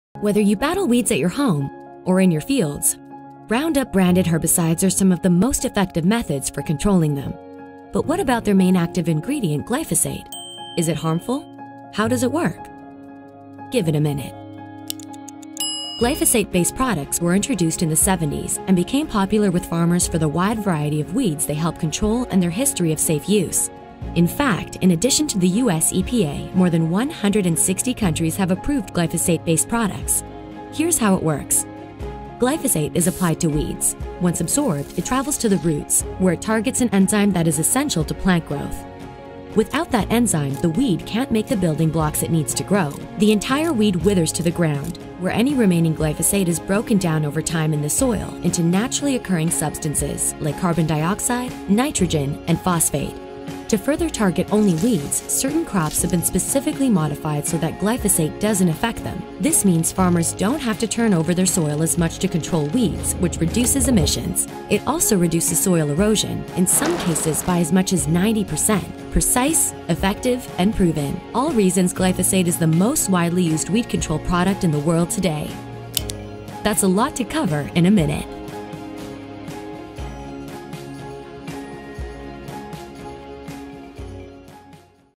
Herbicides | Bayer Give it a Minute
Roundup® branded herbicides are some of the most effective methods that farmers use to control weeds. But is Roundup®—and its main active ingredient, glyphosate—safe? How does it really work? Give it a minute.